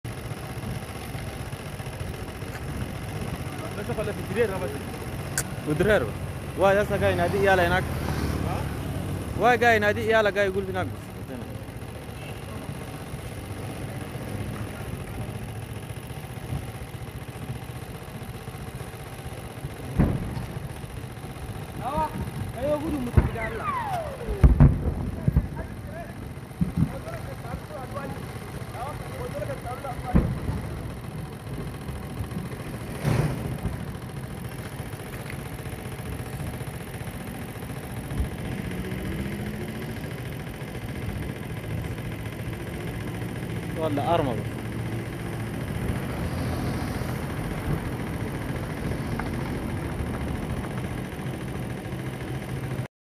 Nine dead, over 40 wounded after explosion at Chad military ammunition depot
An explosion and fire at a military ammunition depot in Chad's capital has killed nine and injured more than 40. The explosions started late Tuesday at the depot in N’Djamena.